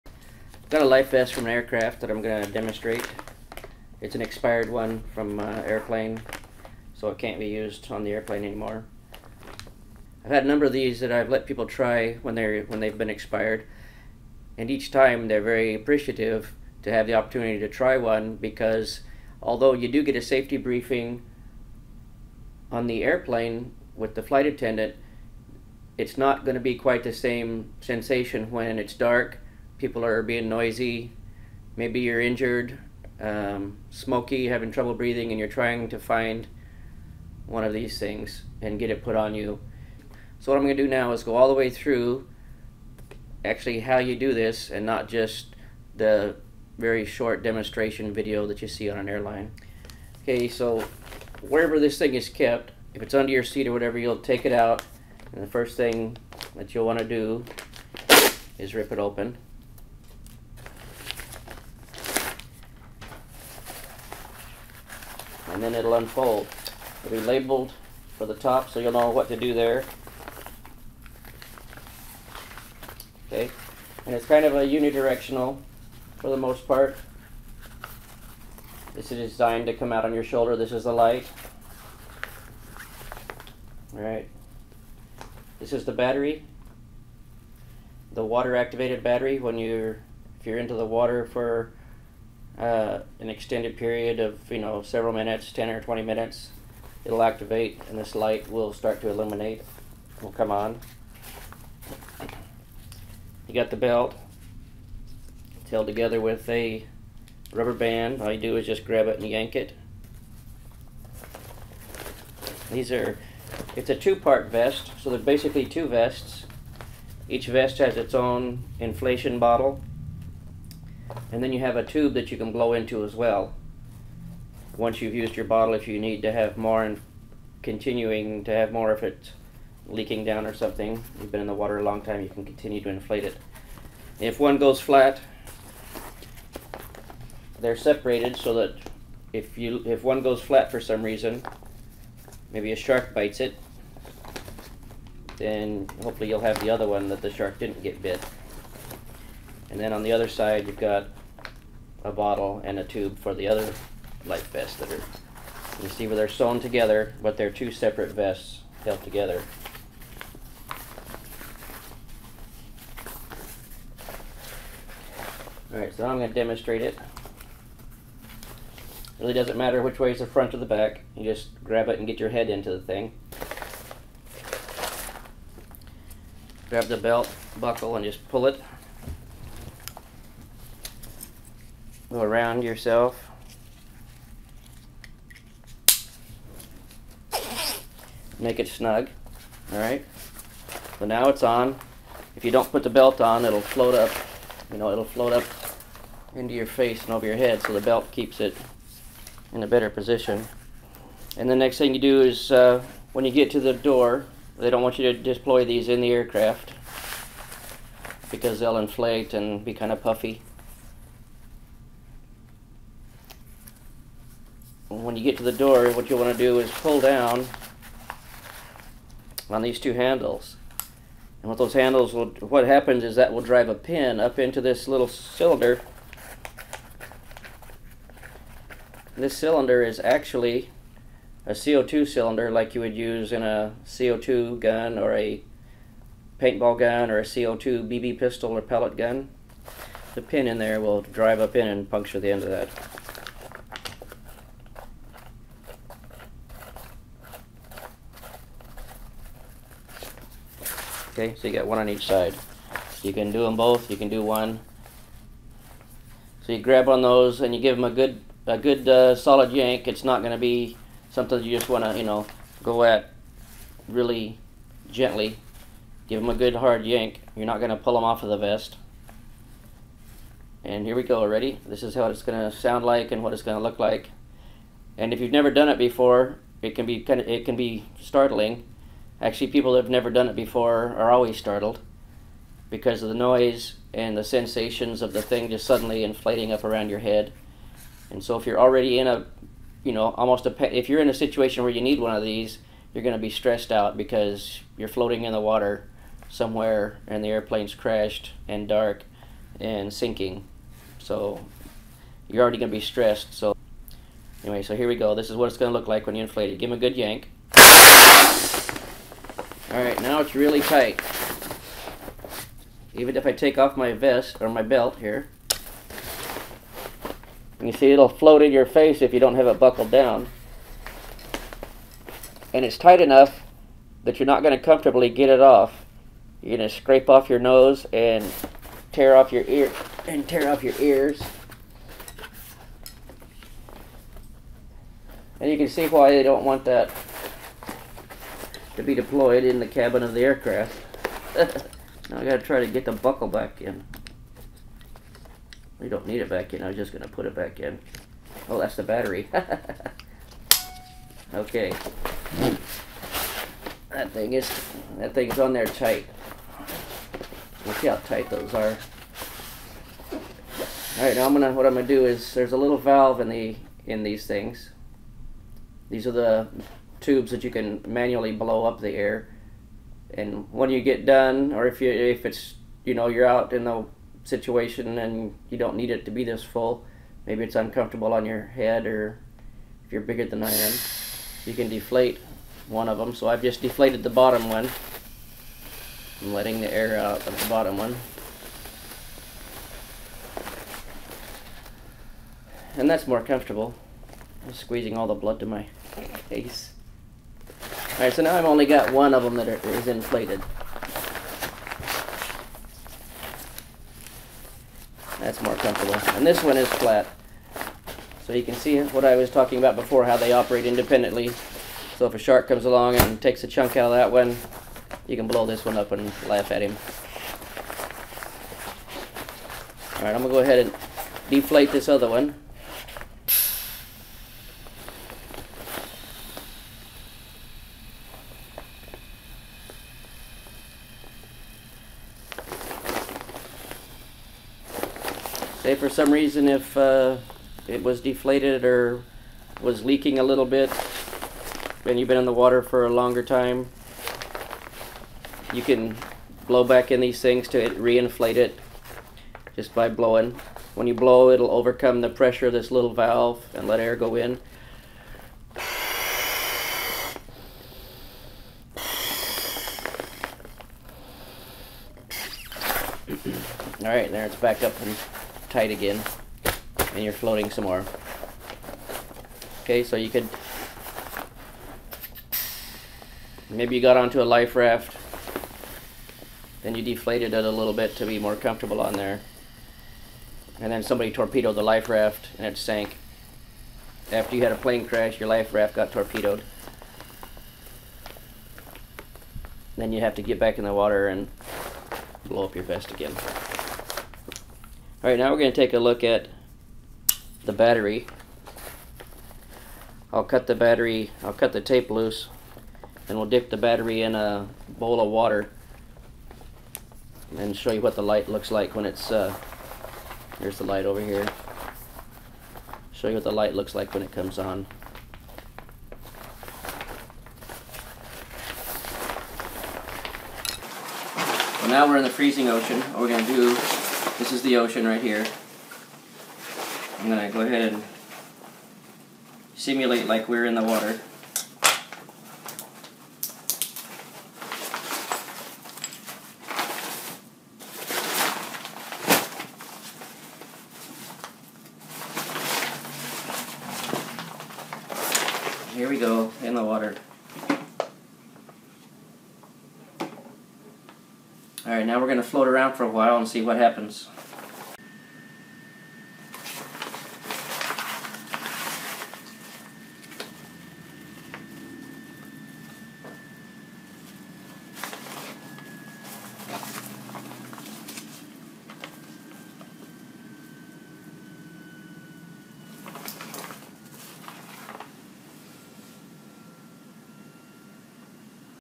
Aircraft Life Vest Full Detailed Demonstration
I have had opportunity from time to time to get a hold of aircraft life vests that have reached their expiration date and can no longer be used aboard the aircraft.  This has provided me with the opportunity to try them out.  I have also had some flight attendants deploy and inflate them.  Even after their training in the airline safety program are startled and surprised when they go through the full process and pull the handles on a live vest and fire the inflation cylinders.  In training they are taught how to put it on and how to demonstrate how to put it on for the passengers but most never actually have the chance to fire a live vest.  Last week one of the flight attendants with 10 years experience fired a life vest for the first time.  It startled her.  In those few seconds she learned so much more about using the life vest that she did not know before.  Always the people that have the opportunity to try one are very appreciative because they did not realize what would actually happen.